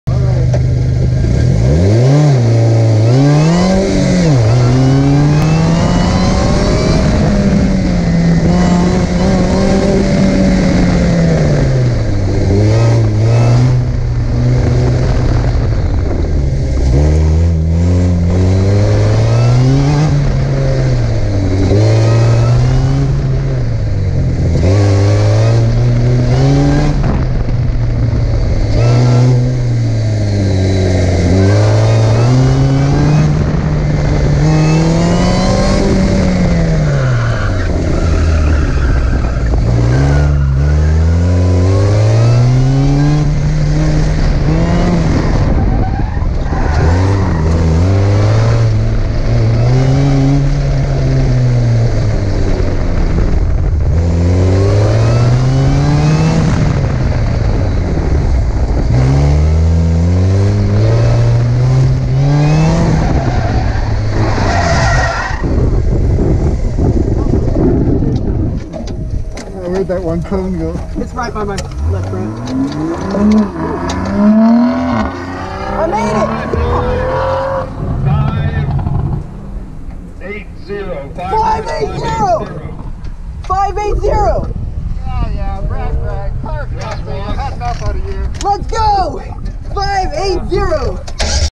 2021 10 09 Autocross
After my 6 laps, My best lap was 60.558 seconds.  I let Tim drive, and here is his second lap.  He finishes the lap completely sideways thru the timing light.  Drives my car it like he stole it.  Enjoy.